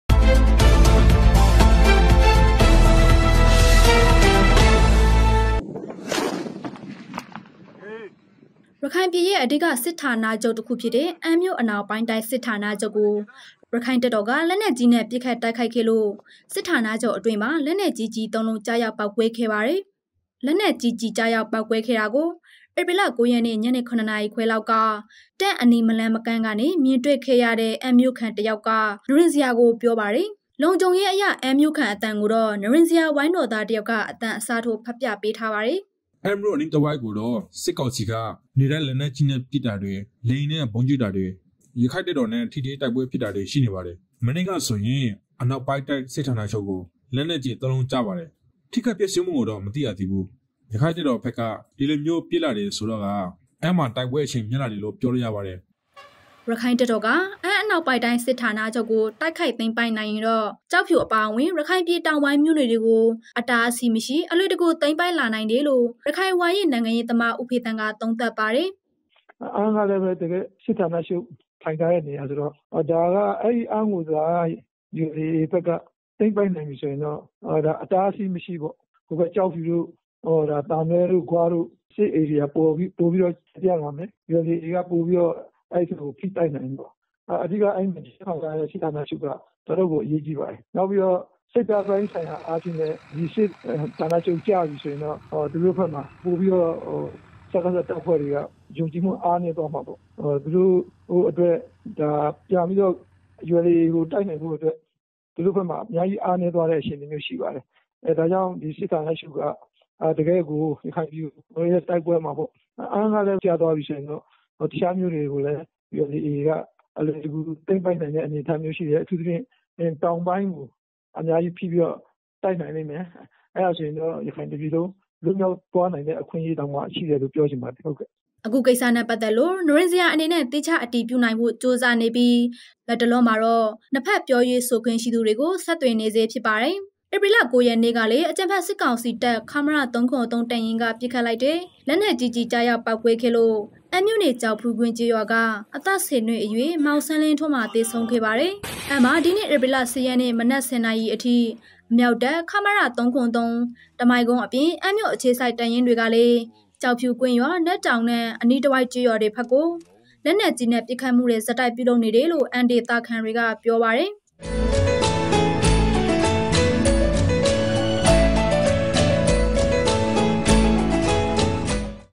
အနောက်ပိုင်းတိုင်းစစ်ဌာနချုပ်ကို ရက္ခိုင့်တပ်တော်က လက်နက်ကြီးနဲ့ တိုက်ခိုက်
ခိုင်ပြည်ရဲ့ အဓိက စစ်ဌာချုပ်တစ်ခုဖြစ်တဲ့ အမ်းမြို့၊ အနောက်ပိုင်းတိုင်းစစ်ဌာနချုပ်ကို ရက္ခိုင့်တပ်တော်က လက်နက်ကြီးနဲ့ ပစ်ခတ်တိုက်ခိုက်ခဲ့တယ်လို့ စစ်ဌာနချုပ်အတွင်း လက်နက်ကြီးကျည် ၃ လုံး ကျရောက် ပေါက်ကွဲခဲ့ပါတယ်။

လက်နက်ကြီး ကျရောက်ပေါက်ကွဲခဲ့တာကို ဧပြီ ၉ ရက်နေ့ ညနေ ၇ နာရီခွဲလောက်က တပ်အနီး မလှမ်မကမ်းကနေ မြင်တွေ့ခဲ့တဲ့ အမ်းမြို့ခံတစ်ယောက်က နိရဉ္စရာသတင်းဌာနကို ပြောပါတယ်။
အဲဒီ အနောက်ပိုင်းတိုင်းစစ်ဌာနချုပ်က အရာရှိအဆင့် စစ်ဗိုလ်တွေအပါအဝင် တပ်ဖွဲ့ဝင် တချို့ဟာလည်း ကိုယ့်နည်းကိုယ့်ဟန်နဲ့ ထွက်ပြေးနေမှုတွေ ရှိနေတယ်လို့ စစ်ရေး သတင်းရင်းမြစ် တချို့ကပြောပါတယ်။